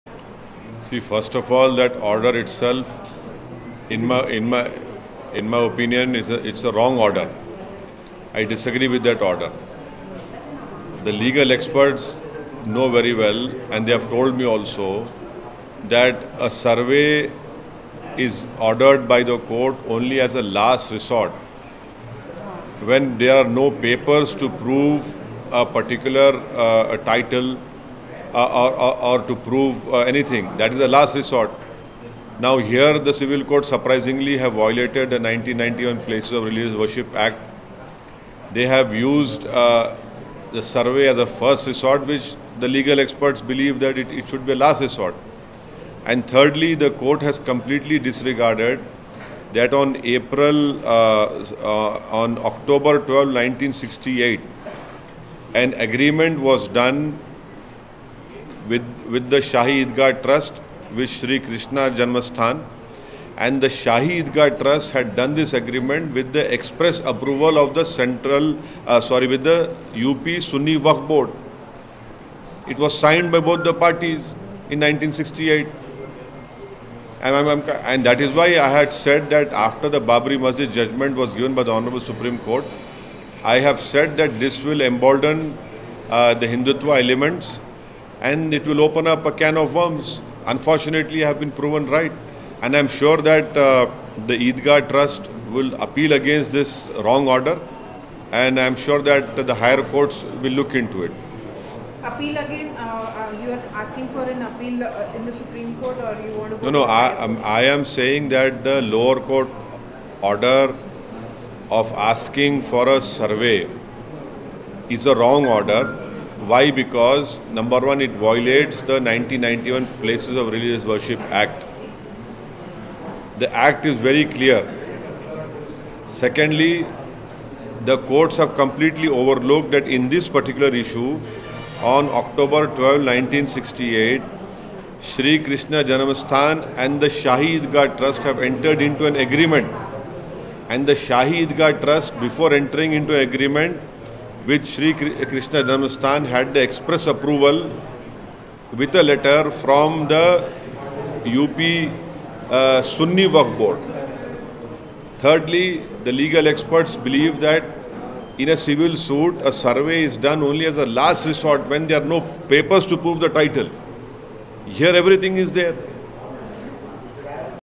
Aimim Chief Barrister Asaduddin Owaisi Today While Interacting With Media At Party
Aimim Chief Barrister Asaduddin Owaisi Today While Interacting With Media At Party Headquarters Darussalam.